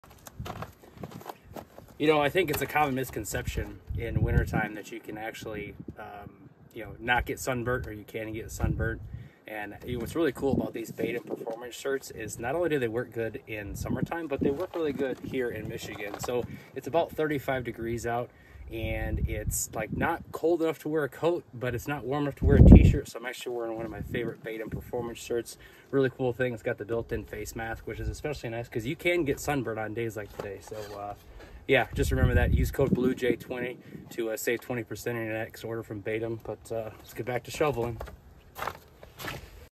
UV sun protection in the winter?
Yes, you can get sun burnt even in the winter time with snow on the ground.

On these mild winter days @baitium performance fishing UV sun protection shirts work great for keeping you protected from the sun and also not too hot.

Head on over to Baitium’s website to check out all of the great products they have to offer.